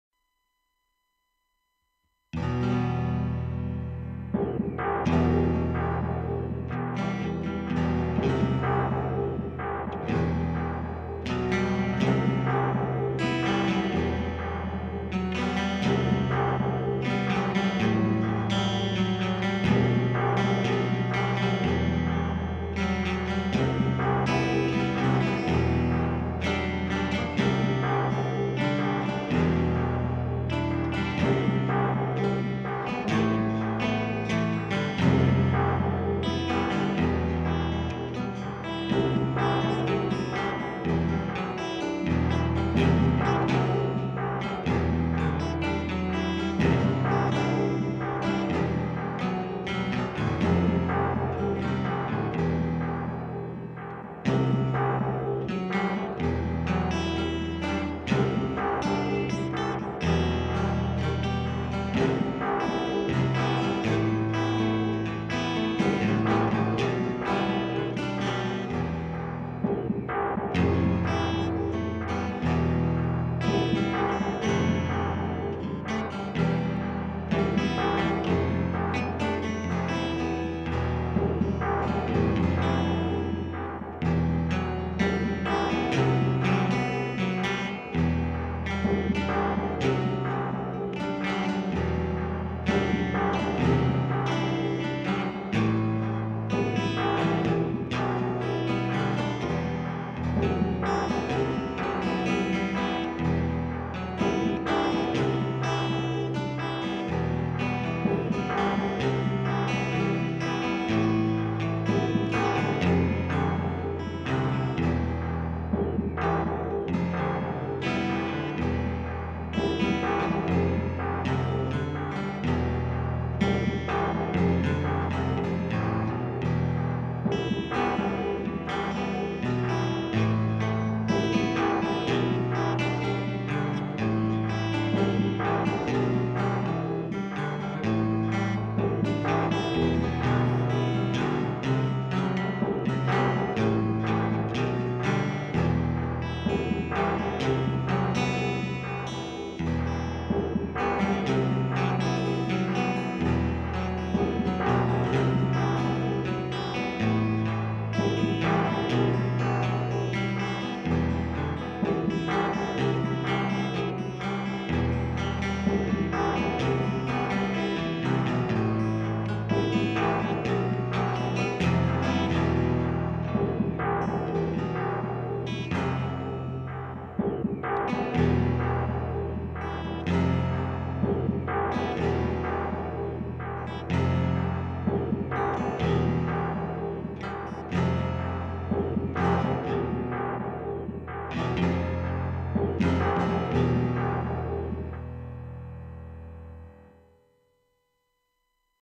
Roland GR-55 w/Line 6 & EMU XL1 Module "Industrial_Piano"
Live improvisation with the Roland GR-55 in parallel with Line 6 Spider Combo amp AND jamming to my EMU XL1 Extreme Lead sound module.

Equipment used:
EMU XL1 Extreme Lead Dance Module